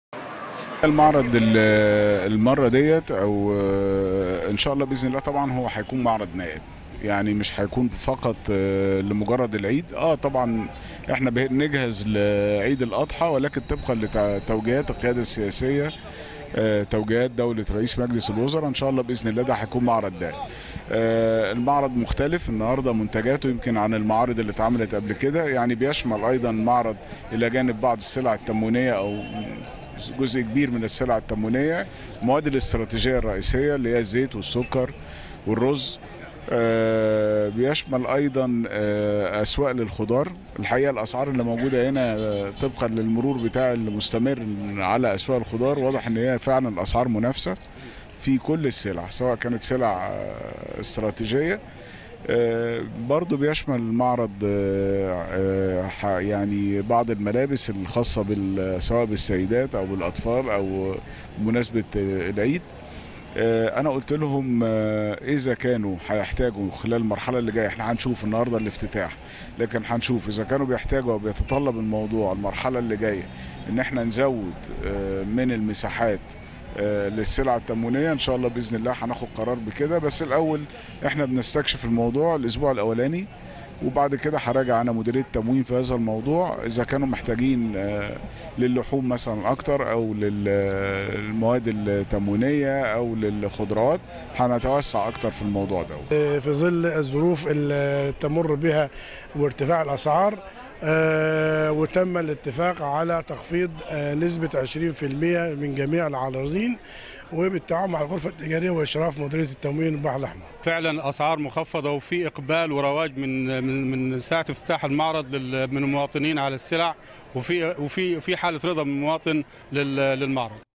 محافظ البحر الأحمر يفتتح معرض "العيد فرحتنا" بأسعار مخفضة للمواطنين
عرضت نشرة أخبار_TeN تقريرًا بعنوان "محافظ البحر الأحمر يفتتح معرض "العيد فرحتنا" بأسعار مخفضة للمواطنين"